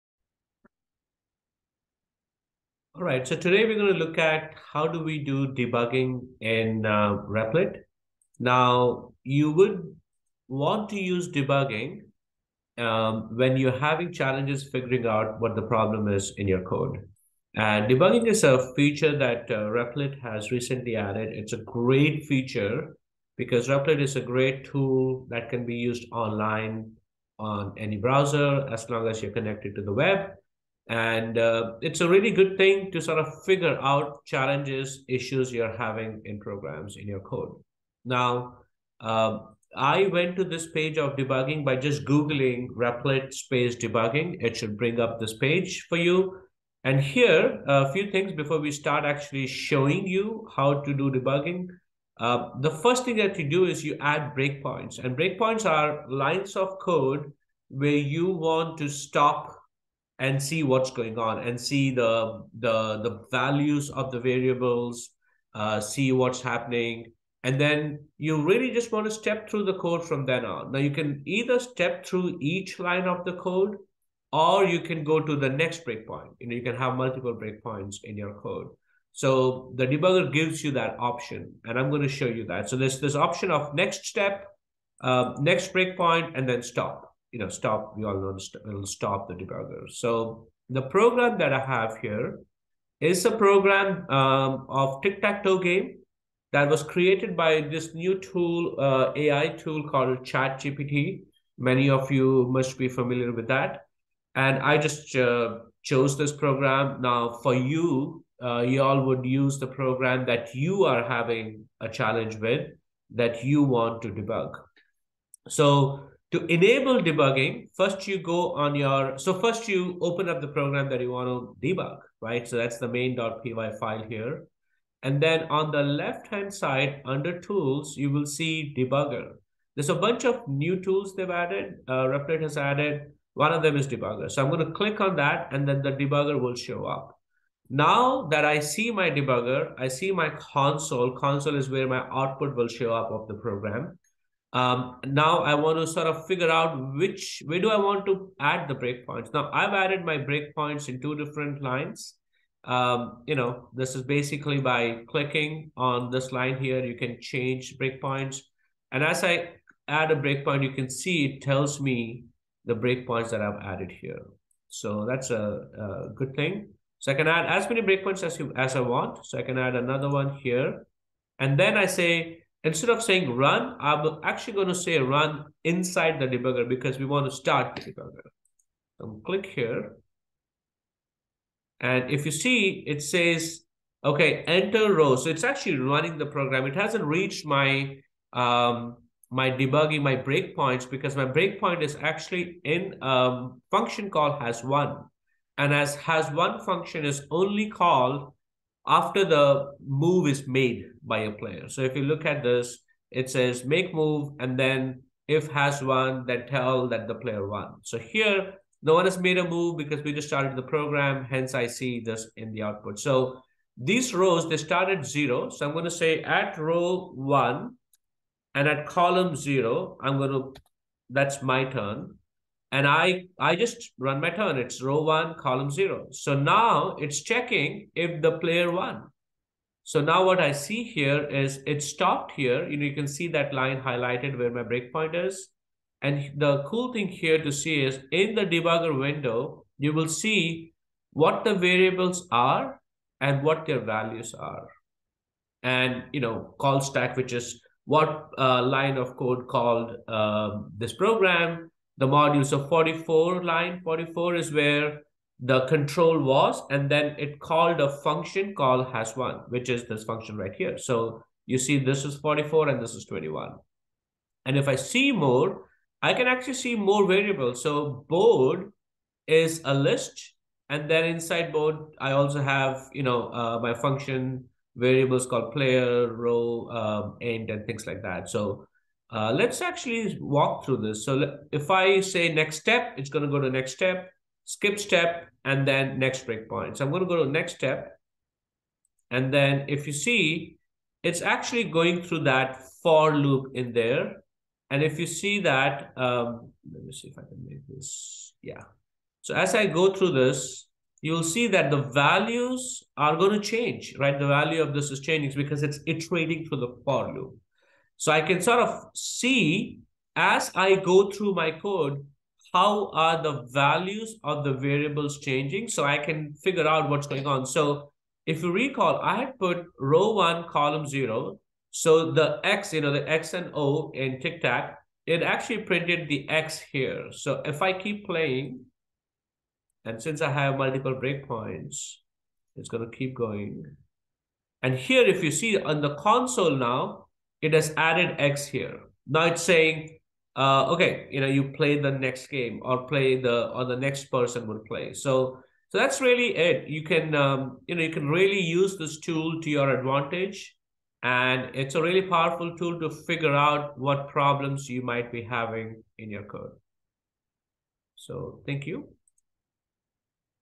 Using Debugger in @replit - finding and fixing issues in your code
Using Debugger in @replit  - finding and fixing issues in your code. We use a tic-tac-toe game created by @ChatGPT-AI to demonstrate debugging. You will debug your code - to identify and potentially fix issues.